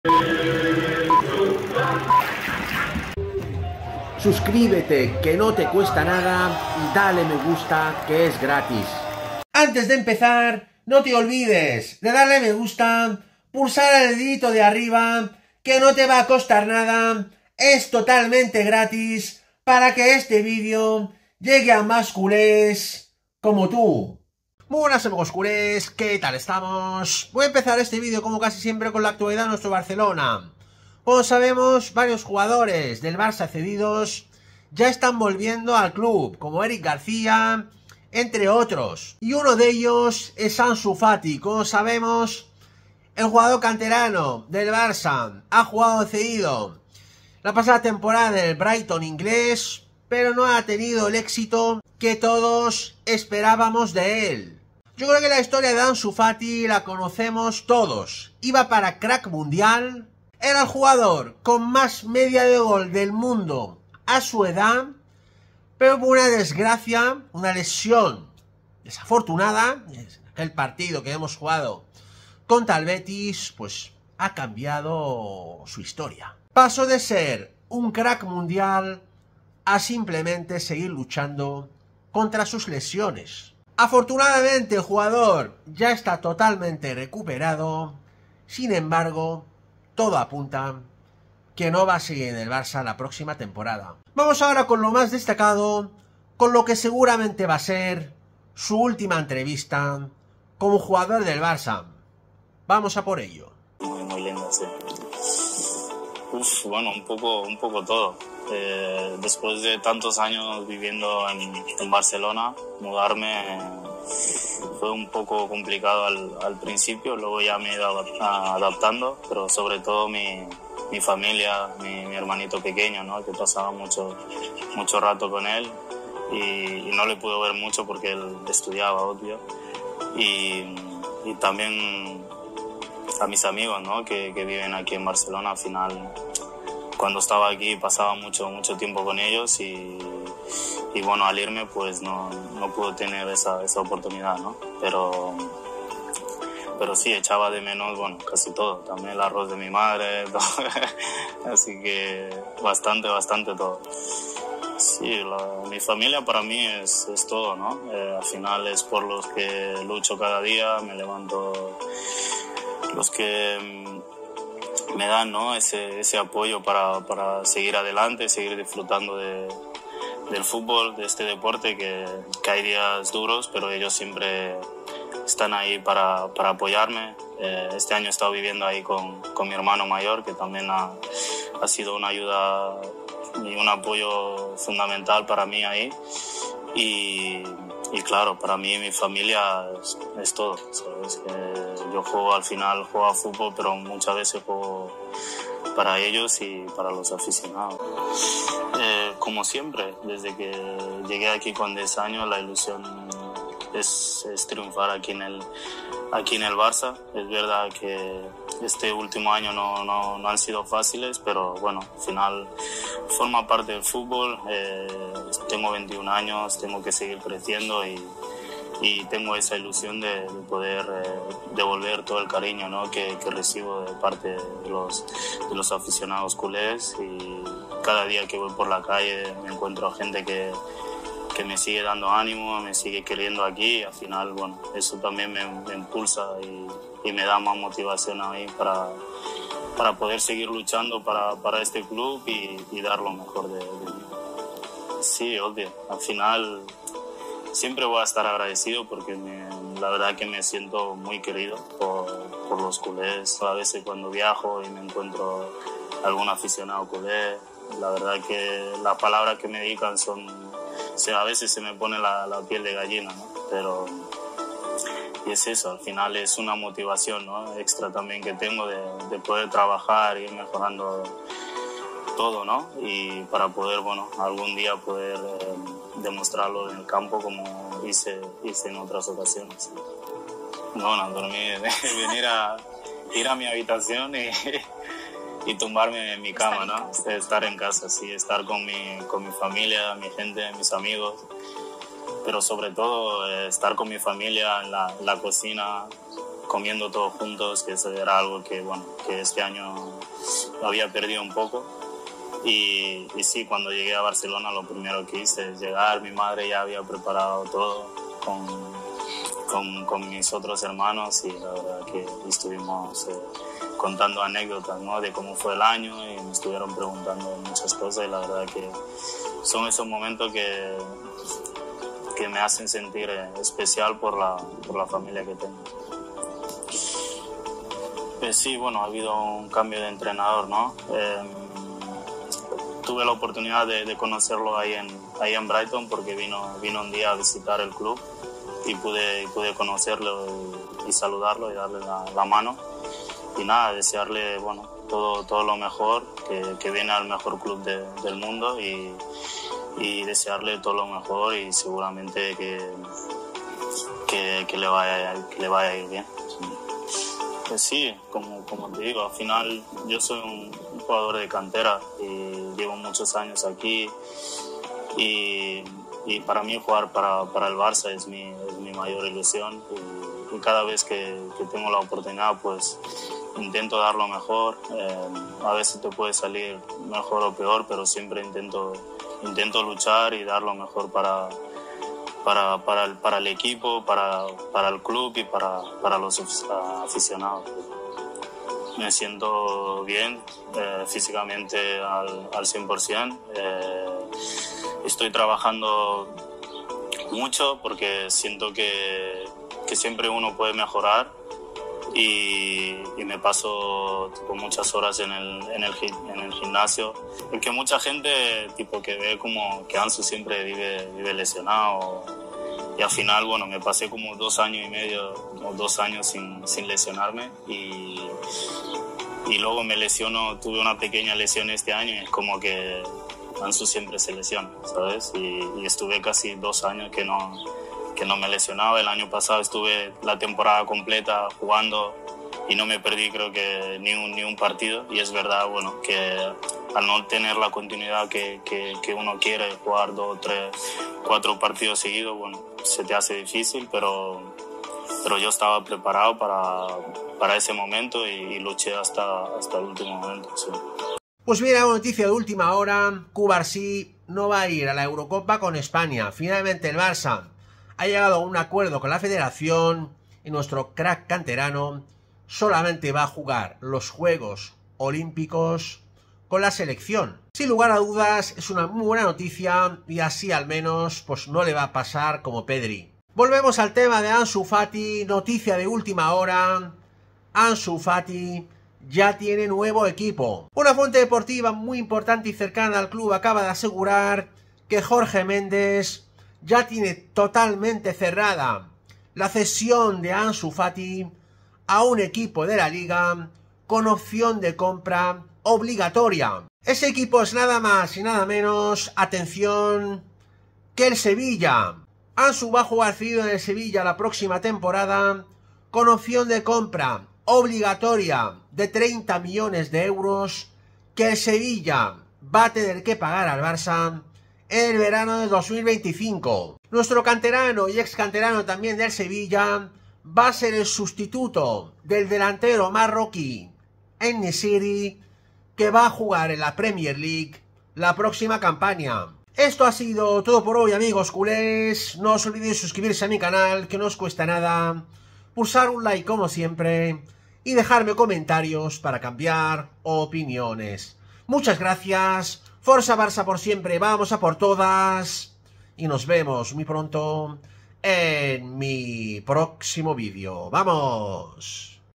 ¡BOMBAZO DE ÚLTIMA HORA! ¡ANSU FATI CIERRA SU FICHAJE POR ESTE BRUTAL EQUIPO!
Ansu Fati cierra su fichaje por un mítico equipo Europeo